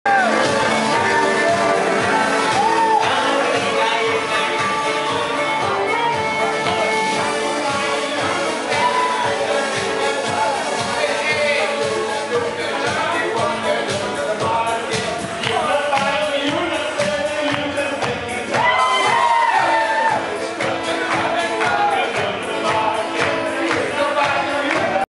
Simply Sfat Simcha & Dancing Ohr Somayach my old yeshiva Sukkos 5773
we need big Salvations, in all ways! Sukkos 5773!